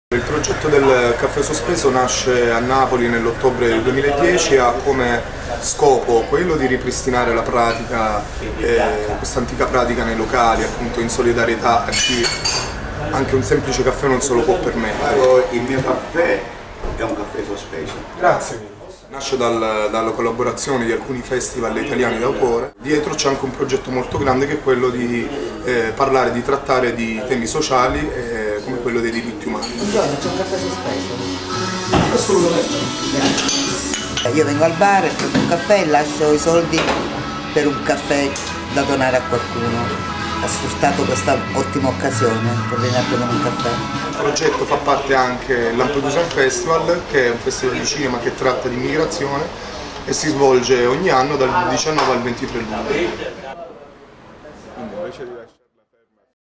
Lampedusa a weekend between Caffe Sospeso and Human Rights
Ascn.Askavusa celebrates the national launch of the network Caffe Sospeso  -outstanding coffee- by various associations and coffee-houses of the peninsula. Askavusa is promoting this old tradition of paying an extra coffee for someone who can get it for economical reasons, on December 10th, World Human Rights Day. The day will close with a blockbuster made in Sicily "La voce del corpo" of the director Luca Vullo.